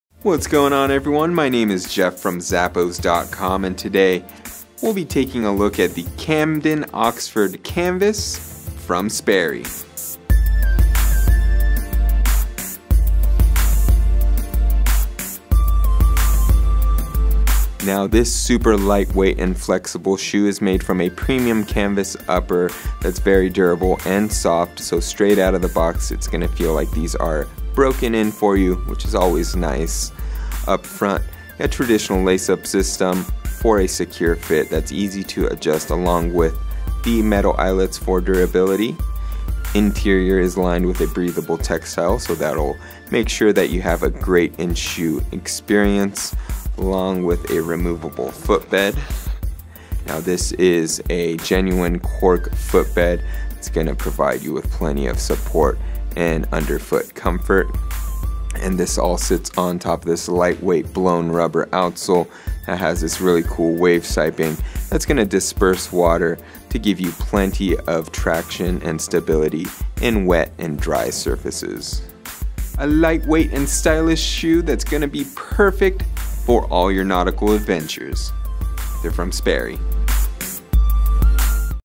Sperry Camden Oxford Canvas SKU: 8981164
The Sperry&reg; Camden Oxford Canvas shoe will upgrade your casual look with its streamline silhouette and refined construction!
Premium canvas upper.
Traditional lace-up style with stretch laces and sturdy metal eyelets for a secure fit.
Genuine cork footbed for all-day underfoot comfort and comfortable ventilation.
Lightweight blown rubber outsole with Wave-Siping&trade; to disperse water for superior traction on both dry and wet surfaces.
Imported.